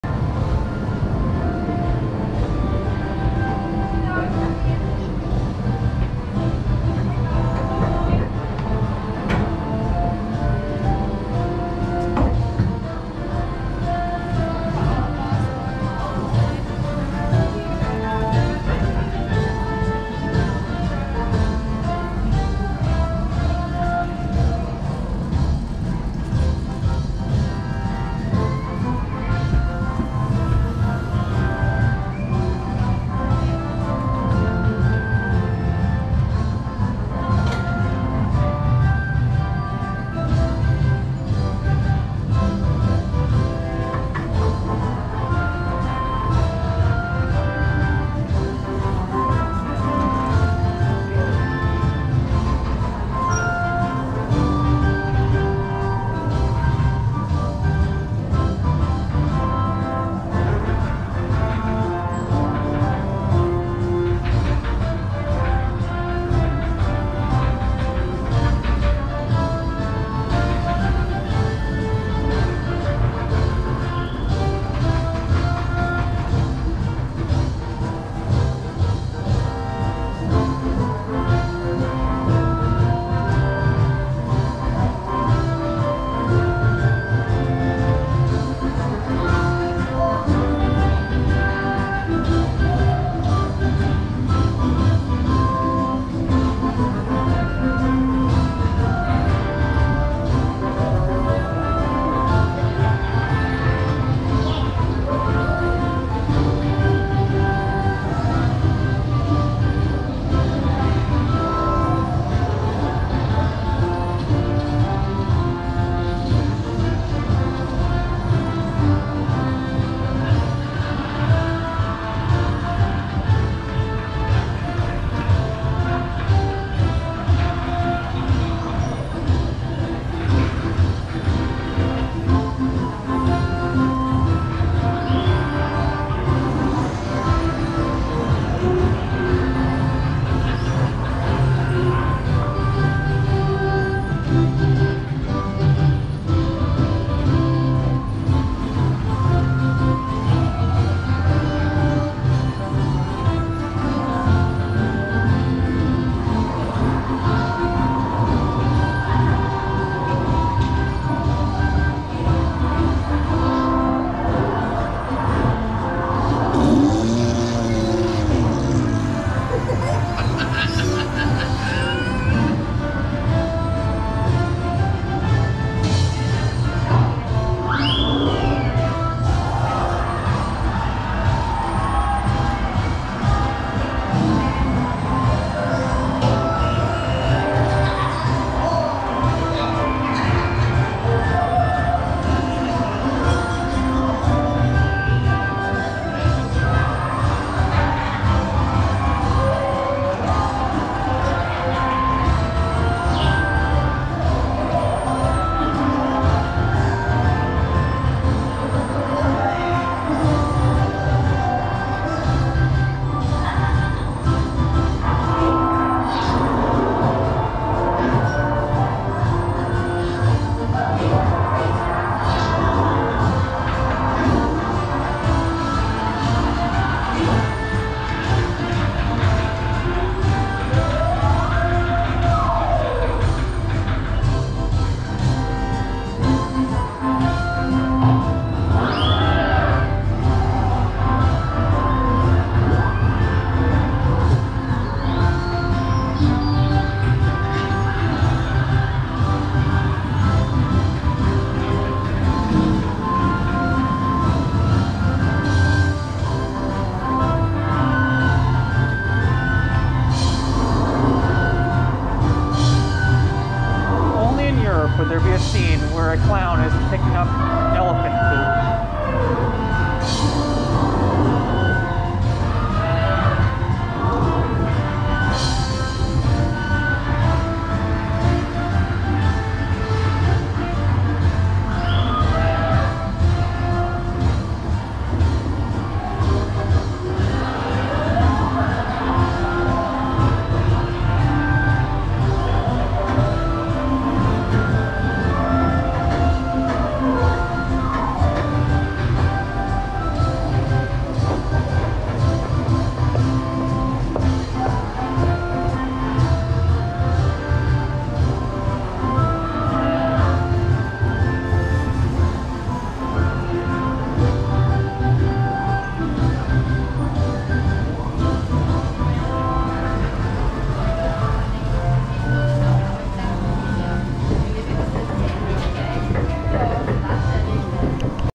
Taikasirkus POV, Linnnanmaki Crazy Clown Dark Ride | Non-Copyright
Taikasirkus is a crazy dark ride at Linnanmaki.  The name translates to Magic Circus and riders are taken past countless scenes with all these crazy clowns.  This ride uses a WGH suspended monorail ride system, features a catchy soundtrack on loop, and some scenes you would never see in America.